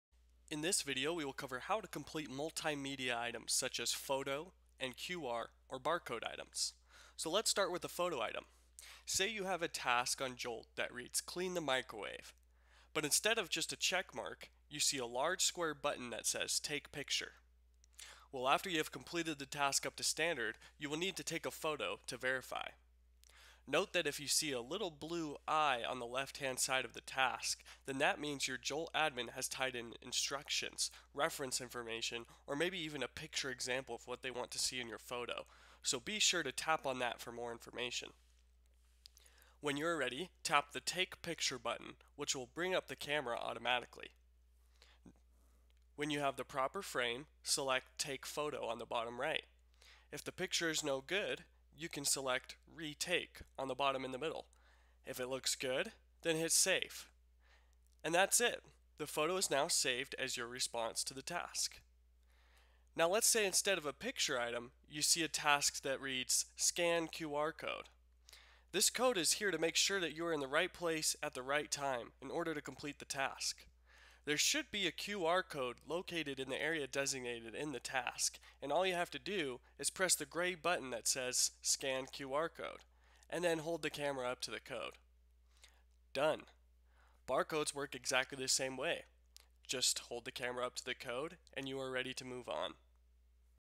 Completing Photo and QR Code Items
This video will show you how to complete Photo and QR Code Items in Jolt.